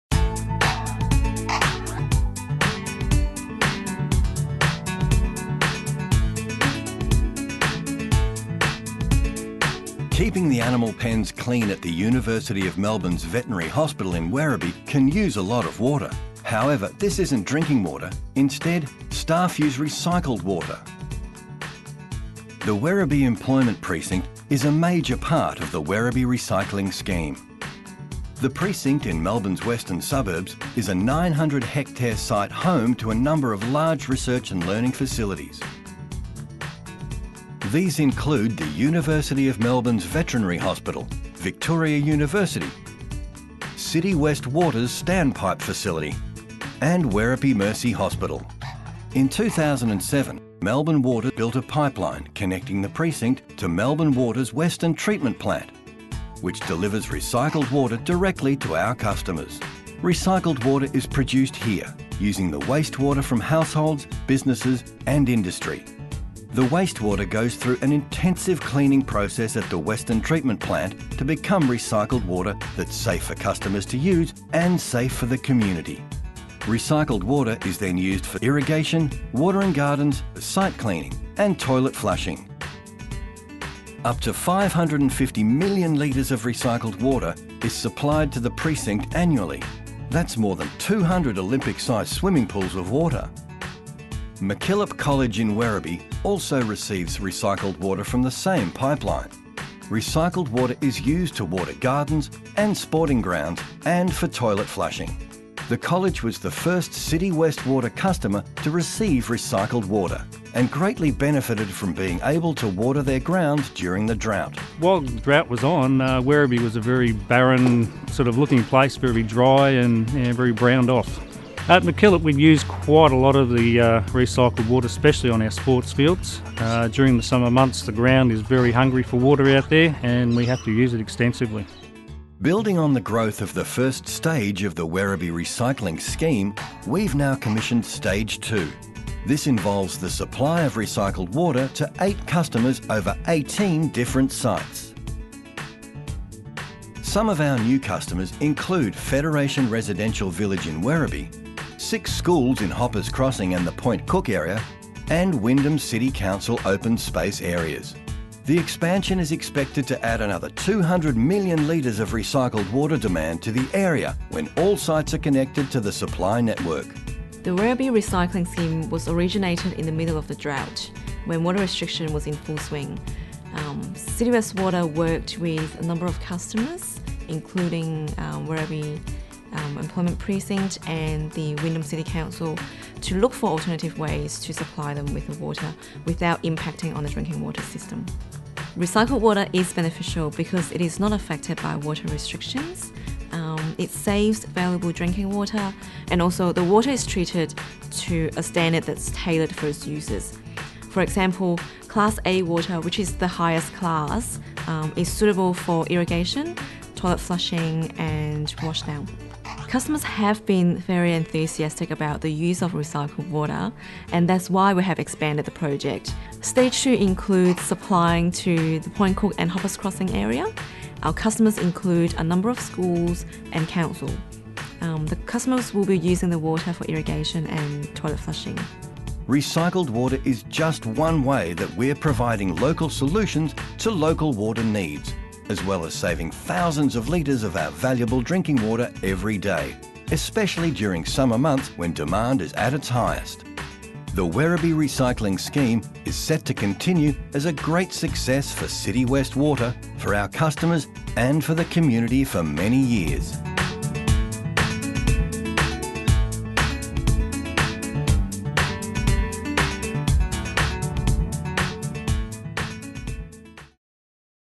City West Water - Werribee Recycling Scheme
In this video, learn how recycled water has been used since 2007 for irrigation, toilet flushing and wash down by industry, businesses and Wyndham City Council in the Werribee and Point Cook region. The video explains how the Werribee Recycling Scheme works and how Stage 2 of the project will increase supply to 18 new customers.